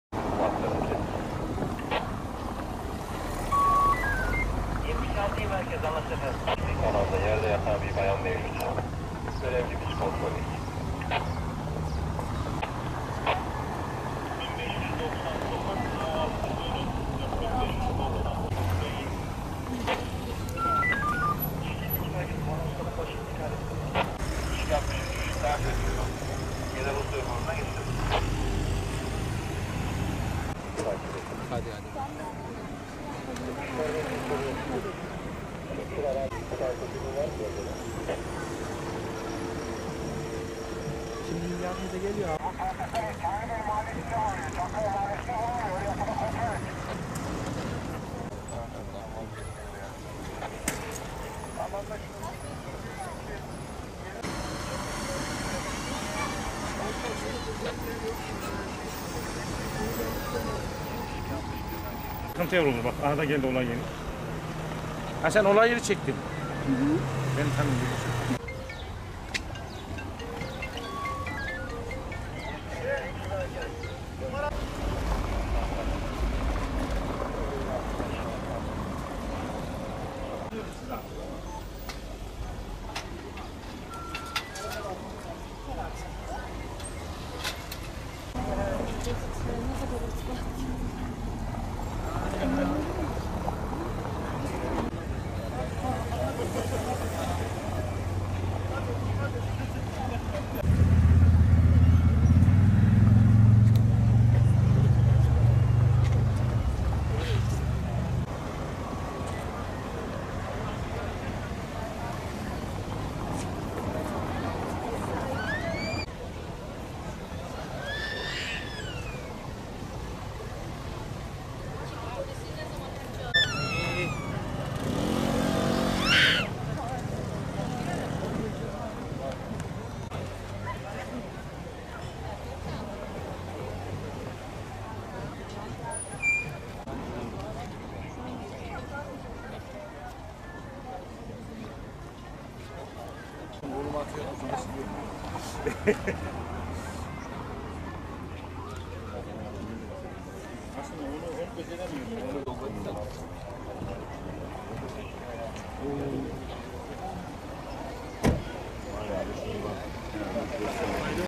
Park halindeki kiralık otomobilde cesedi bulundu
Antalya’da Konyaaltı sahili yakında kiralık park halindeki otomobilin içerisinde bir kişinin cansız bedeni bulundu. Meraklı vatandaşlar ise olup biteni adeta film izler gibi izledi.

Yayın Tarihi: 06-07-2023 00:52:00